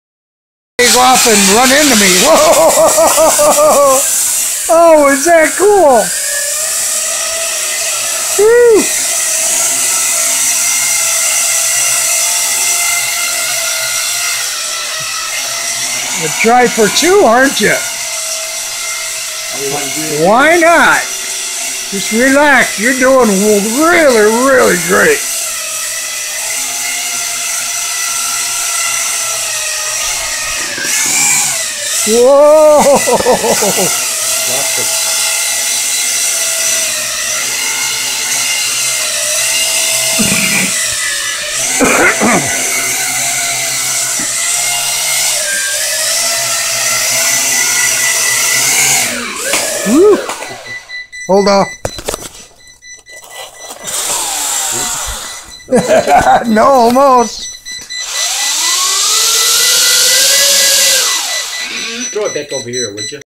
Ronnies new Quad 2
This is really a nice quad. has a 2s battery and will be a fun flyer outside this spring..